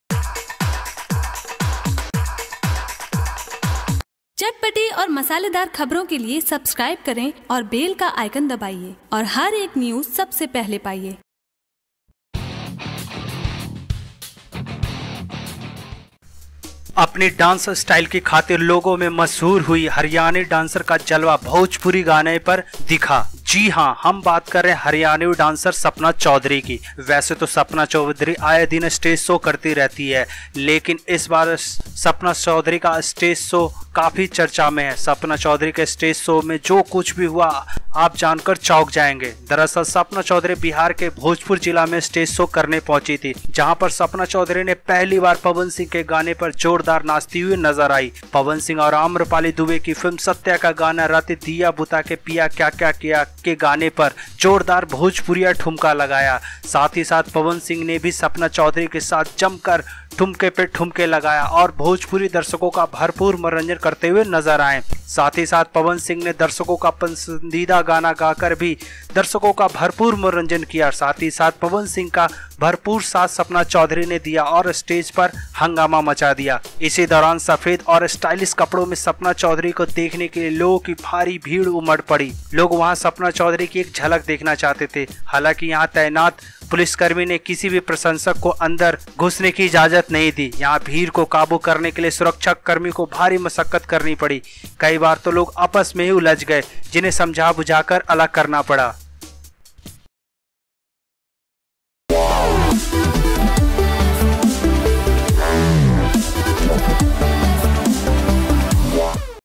पवन सिंह के गाने पर सपना चौधरी का भोजपुरिया ठुमका पटना में II Sapna & pawan
पवन सिंह के गाने पर सपना चौधरी का भोजपुरिया ठुमका पटना में